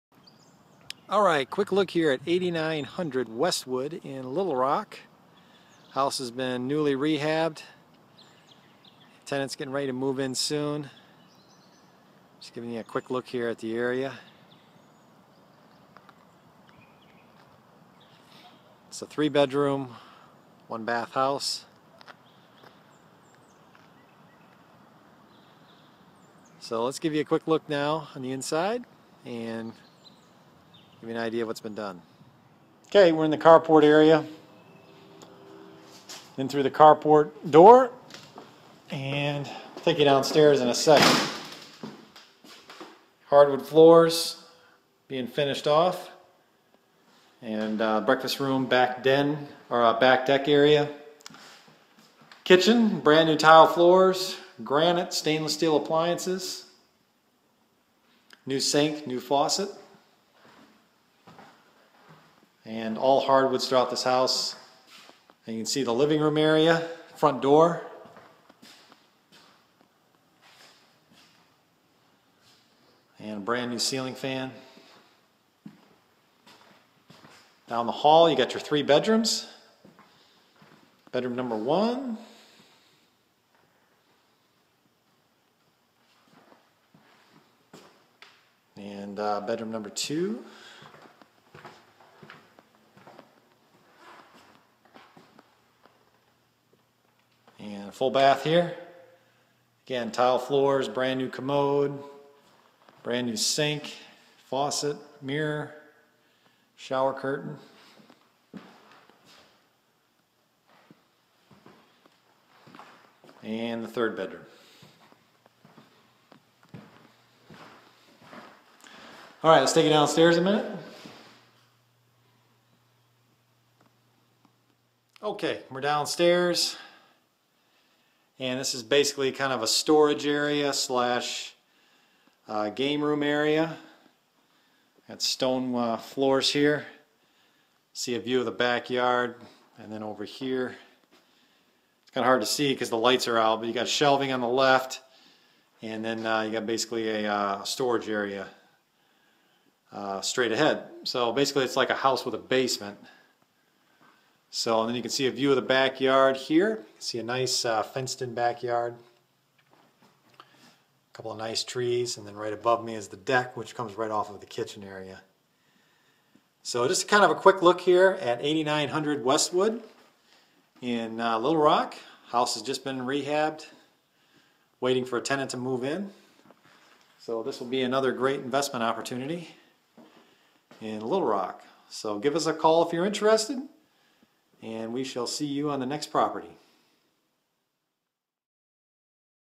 8900 Westwood. Little Rock, Arkansas 72204
After rehab video- 3 bedroom, 2 bathroom, 1,170 square foot house located in the Westwood Pecan area of Little Rock.  Brand new roof, ceiling repair, interior and exterior paint, granite counter tops in kitchen, tile floors in kitchen and bathroom, beautiful hardwood floors, and many other extras that tenant will be proud to call home!  Pending tenant move-in at $925/month.